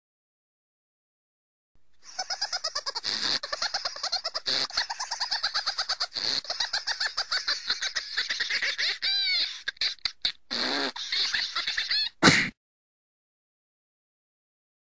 Wonder if Stinch laughing will get him more than 9000 views?
Finally got back to animating.

To prove it, I gave Stinch the giggles to show that the Hollow Fields franchise needs more attention!